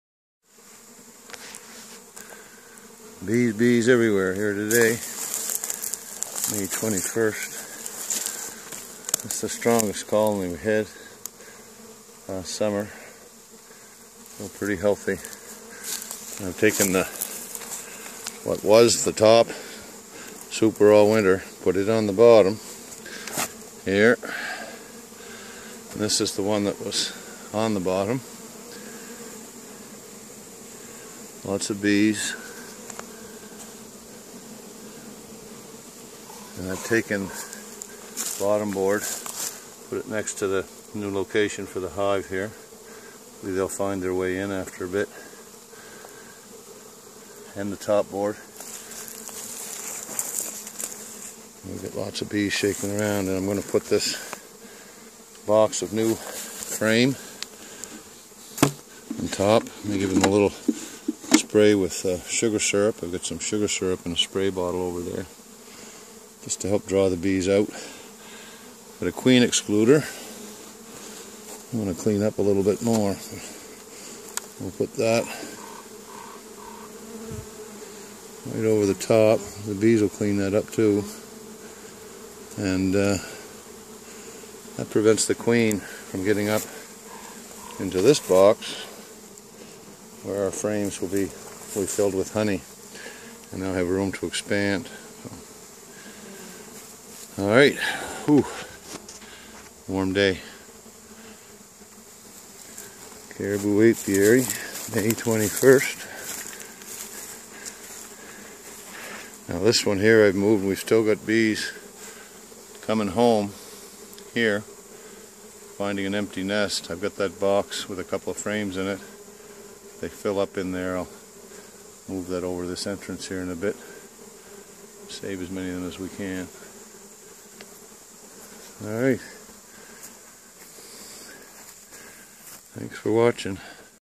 Caribou Bee Yard - Preparing for Blossom 1 May 21st 2015
Posting 3 small clips today of our Honey Bee apiary in Caribou, Nova Scotia, Canada.

All 4 of our colonies have made it through the winter in great shape!

There is still some honey within from last season, so we left enough with the bees to keep them fed and alive through this last winter and long cold spring we have been experiencing here.

Without room to expand, there is a danger of the colony swarming to another nesting site. So, we are adding another super with empty frames for our bees to build out with wax and store honey in.

Michael MacMaster
Caribou, NS
Canada

If you might like to support our apiary, please use our Go Fund Me link here and thank you very much!